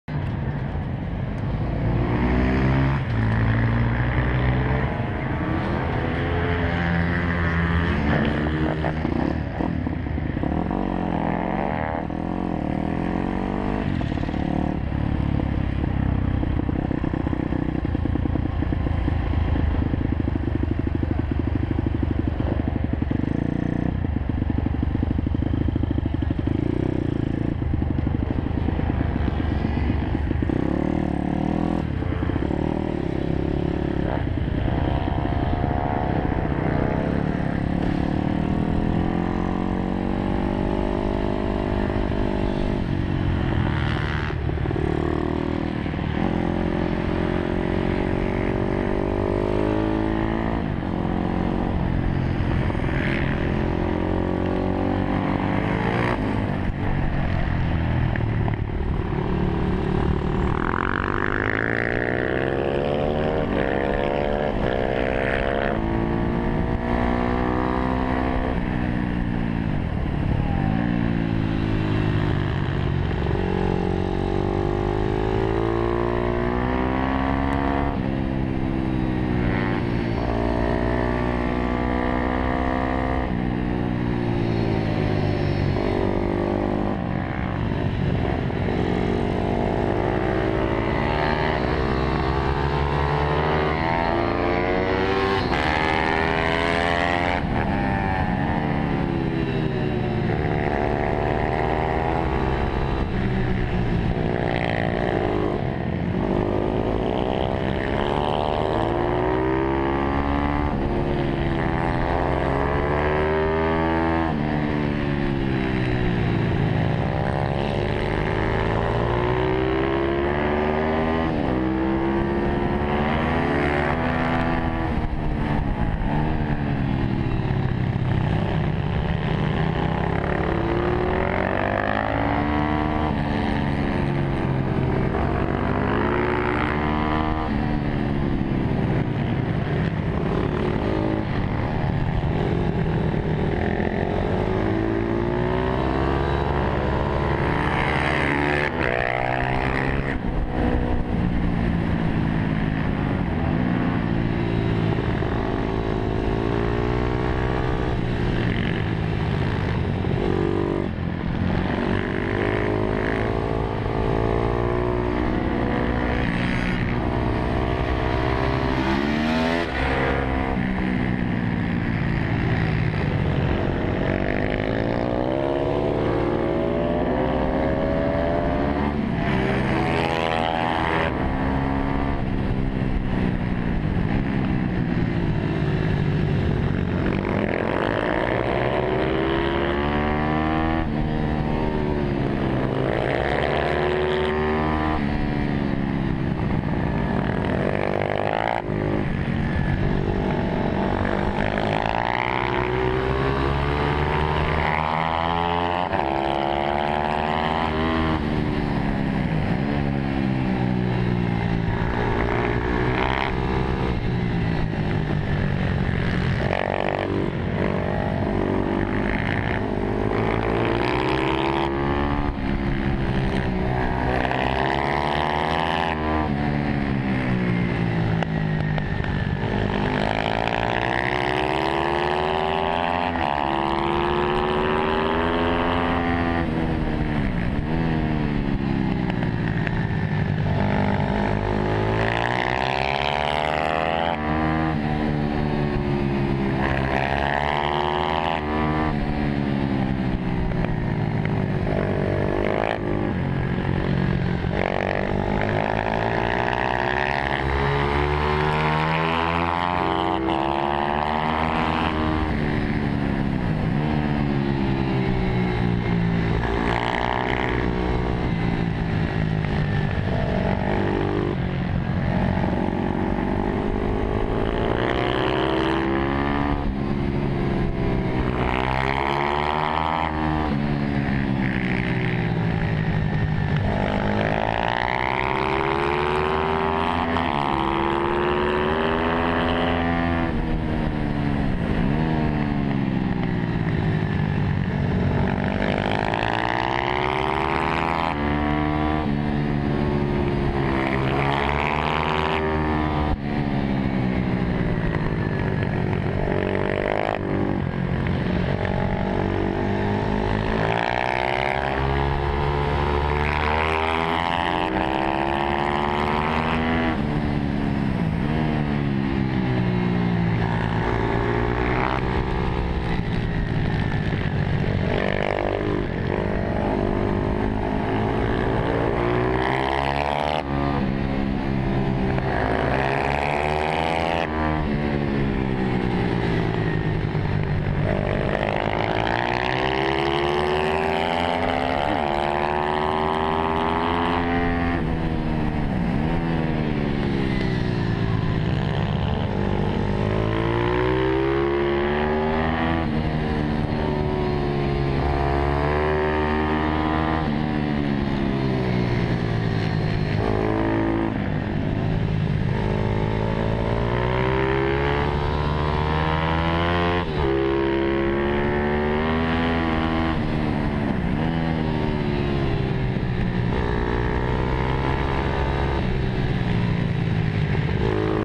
pista azzurra drake vs sico 87 mini gp
un turno con sico 87 e la sua mini gp! bel motore ha sotto